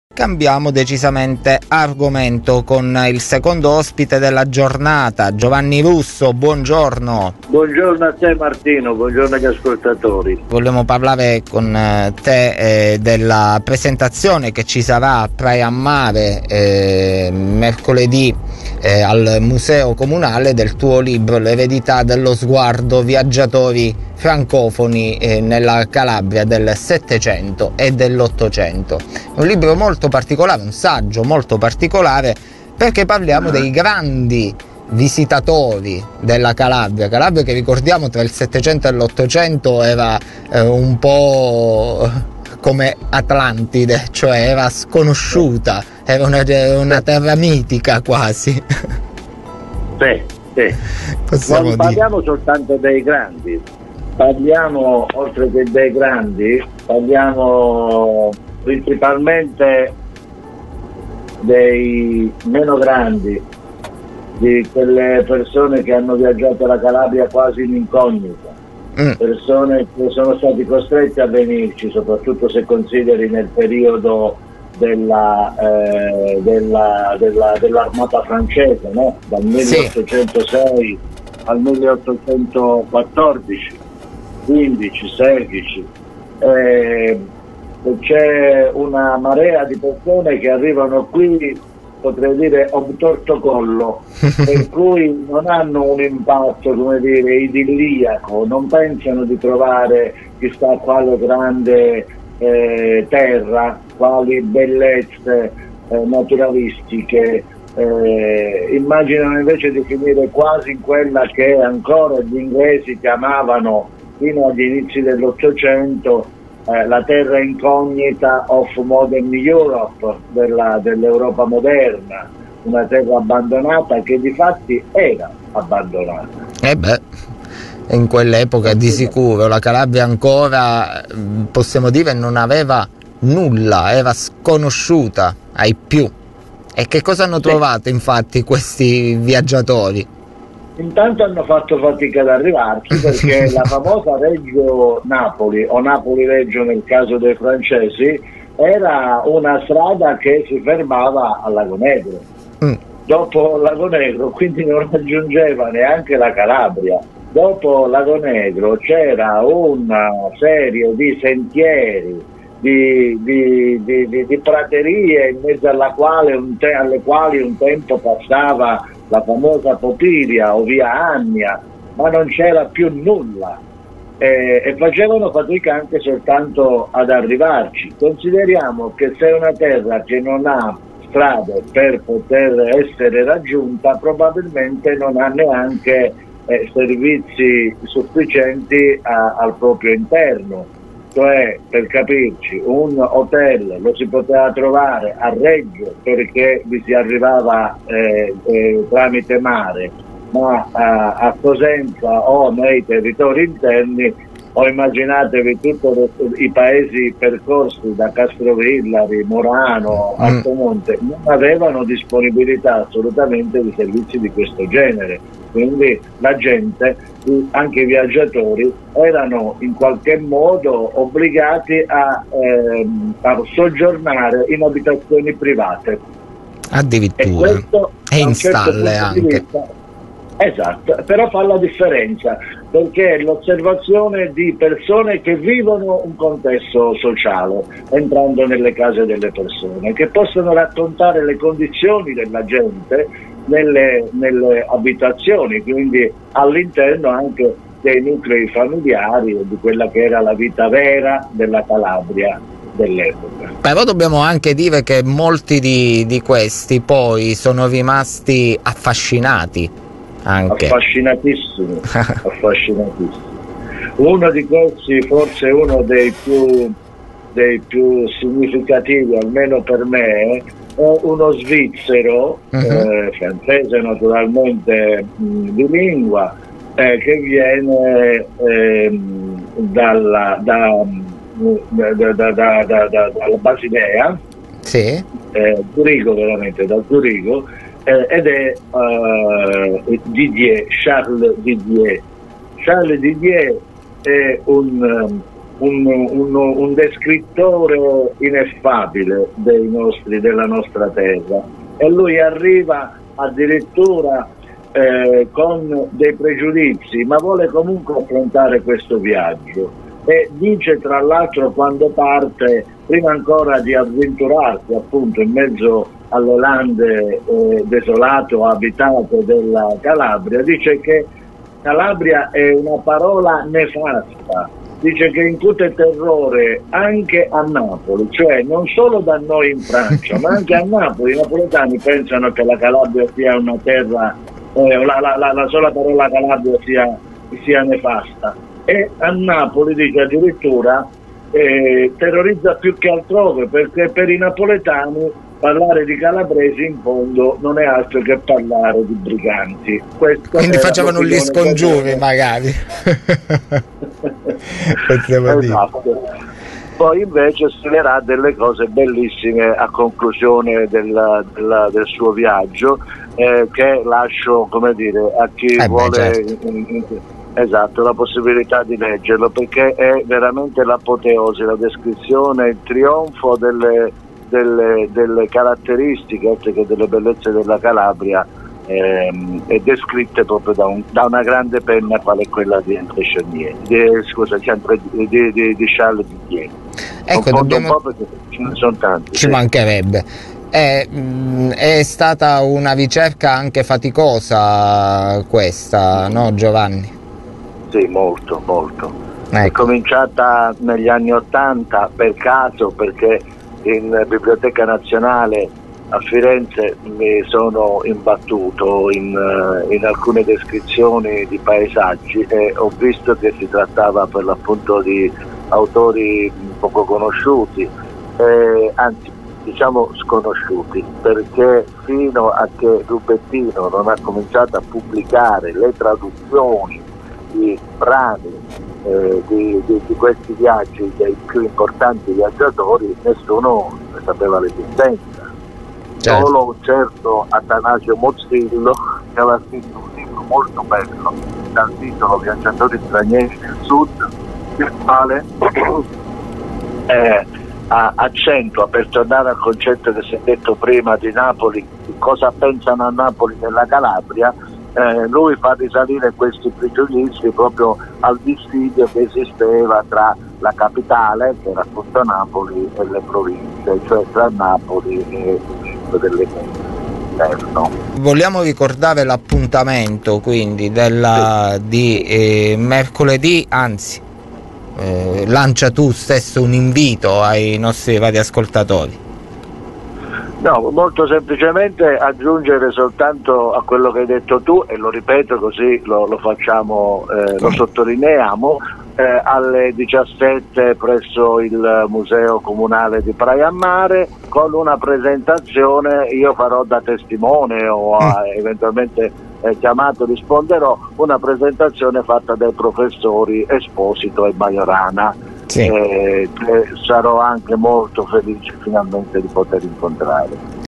L'eredità dello sguardo. Il libro di Giovanni Russo al Museo comunale di Praia a Mare
La presentazione del libro si terrà mercoledì 22 gennaio, alle ore 17, al Museo comunale di Praia a Mare. Relatori: i professori Enrico Esposito e Anna Maiorana.